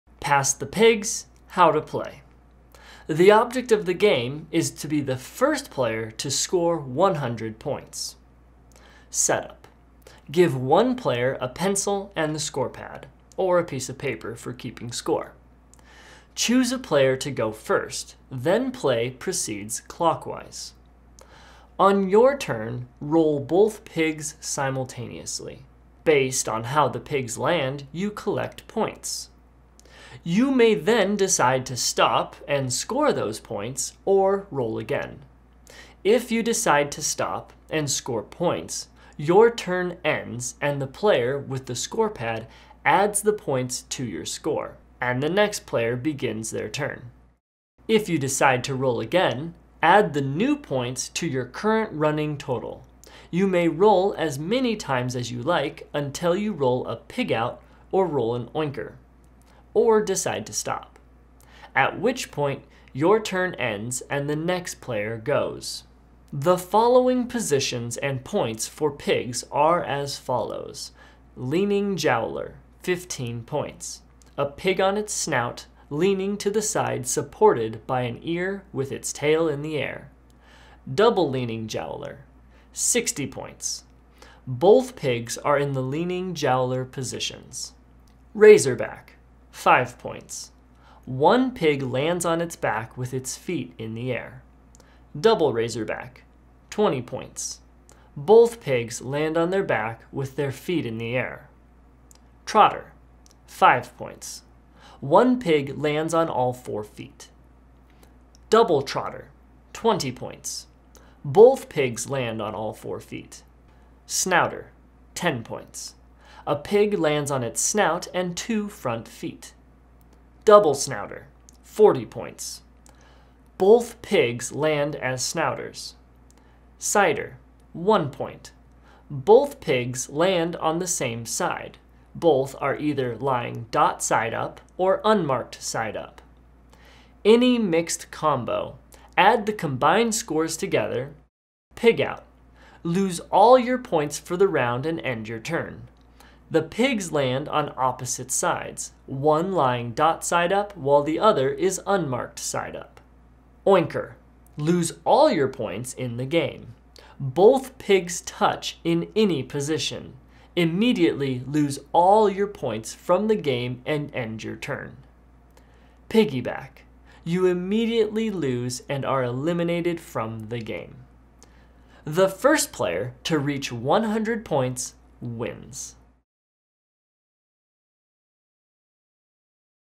How to play Pass the Pigs
Learn the rules to the board game Pass the Pigs quickly and concisely - This visually rich video has no distractions, just the rules.

Don't own the game?

RULES:

The object of the game is to be the first player to score 100 point. Setup. Give one player a pencil and the scorepad, or a piece of paper for keeping score. Choose a player to go first, then play proceeds clockwise. On your turn, roll both pigs simultaneously. Based on how the pigs land you collect points. You may then decide to stop and score those points or roll again. If you decide to stop and score the points, your turn ends and the player with the scorepad adds the points to your score, and the next player begins their turn.

If you decide to roll again, add the new points to your current running total. You may roll as many times as you like until roll a “pig out” or roll an “oinker” or decide to stop. At which point your turn ends and the next player goes.

The following positions and points for pigs are as follows:
Leaning Jowler. 15 points. A pig on its snout, leaning to the side supported by an ear with its tail in the air.

Double Leaning Jowler. 60 points. Both pigs are in the leaning jowler positions.

Razorback. 5 points. One pig lands on its back with its feet in the air.

Double razorback. 20 points. Both pigs lands on their back, with their feet in the air.

Trotter. 5 points. One pig lands on all four feet.

Double Trotter. 20 points. Both pigs land on all four feet.

Snouter. 10 points. A pig lands on its snout and two front feet.

Double Snouter. 40 points. Both pigs land as snouters.

Sider. 1 point. Both pigs land on the same side.  Both are either lying dot side up, or unmarked side up.

Any mixed combo. Add the combined scores together. A single sider does not count for any points.

Pig out. Lose all your points for the round and end your turn. The pigs land on opposite sides. One lying dot side up while the other is unmarked side up.

Oinker. Lose all your points in the game. Both pigs touch in any position. Immediately lose all your points from the game and end your turn.

Piggy Back. You immediately lose and are eliminated from the game.

The first player to reach 100 points wins.